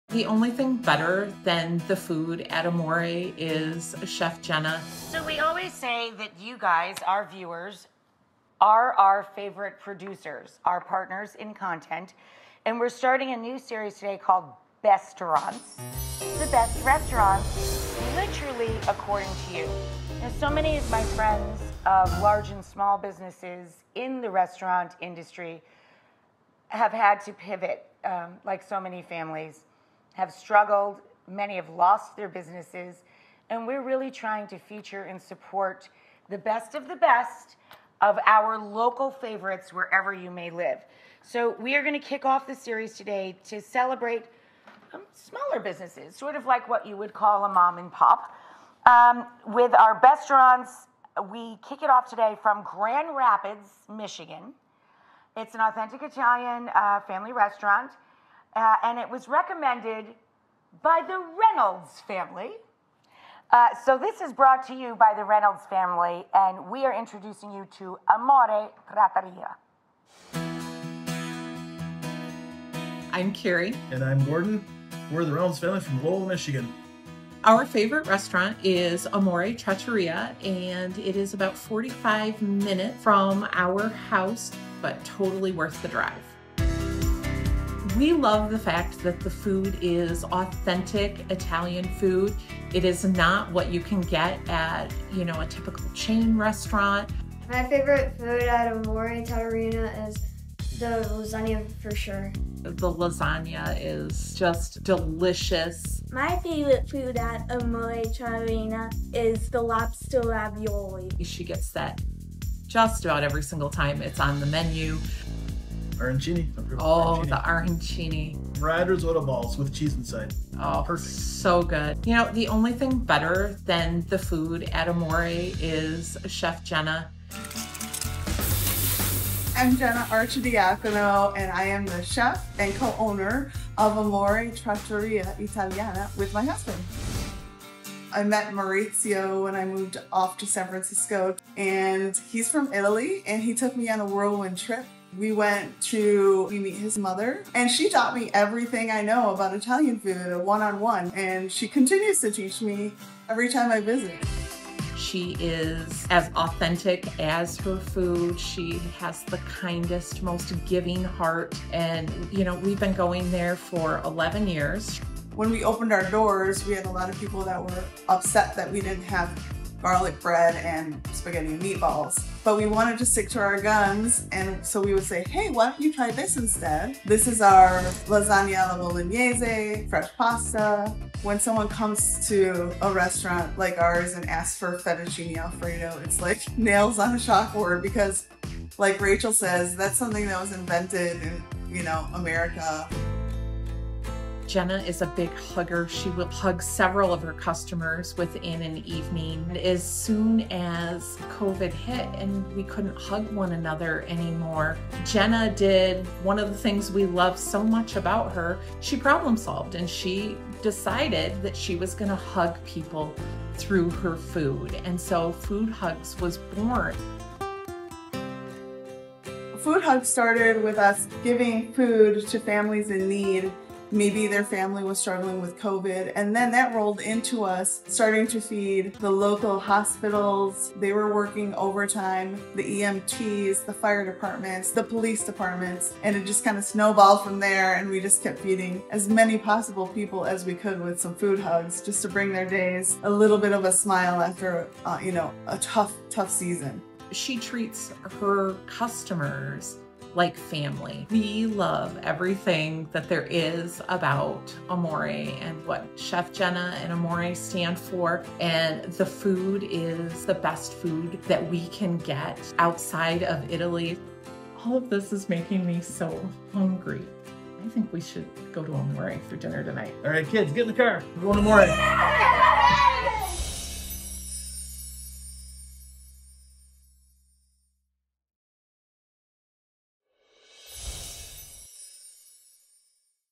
At Grand Rapids' Amore Trattoria, You Come for the Authentic Italian Food, But Stay for Chef Jenna
Amore Trattoria is this Michigan family's favorite neighborhood restaurant because of the wonderful food + the woman behind it.